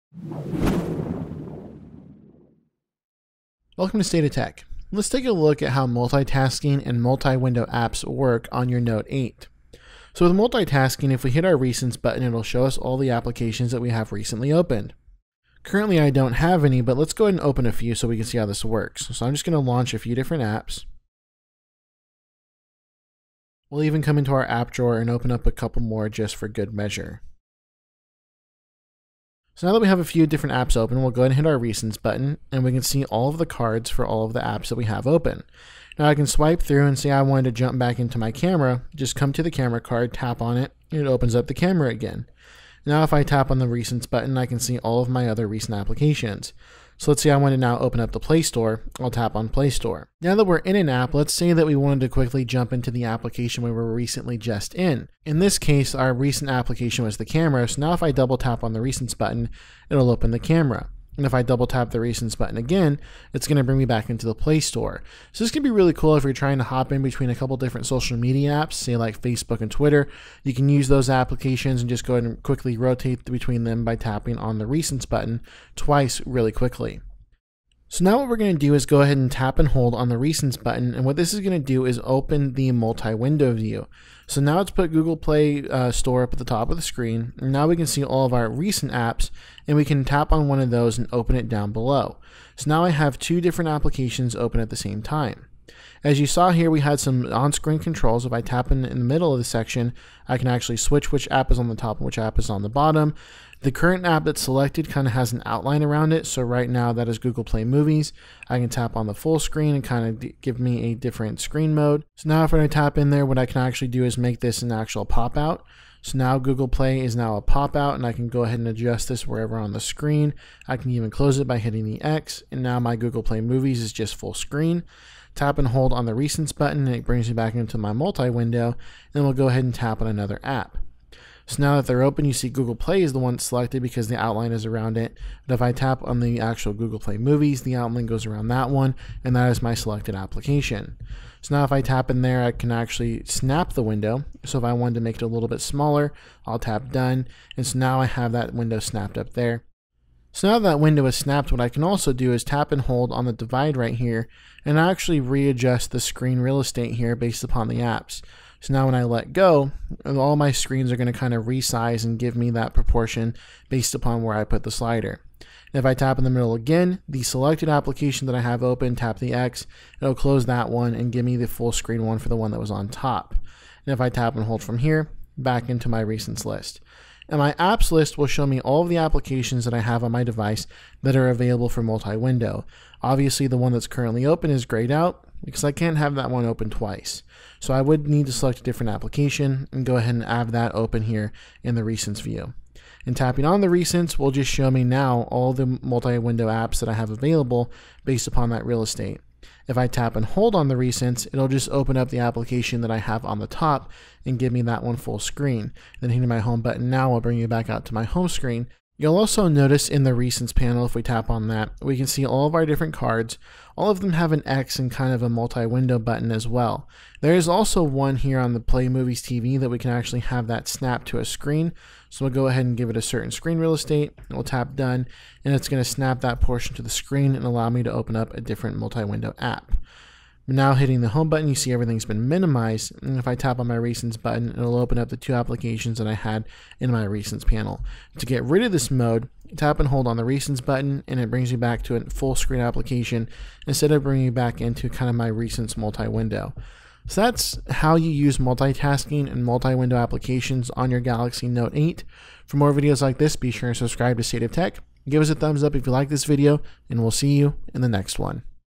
Galaxy Note 8 - How to Utilize the Multitasking Feature and Multi-Window Apps
The Samsung Galaxy Note 8 is more than just a smartphone, it is a productivity powerhouse! There are several cool features that enhance your overall experience as well as your ability to multitask. In this video, learn how to use the Multitasking feature and how to use the Multi-Window App features of your Samsung Galaxy Note 8 Phone.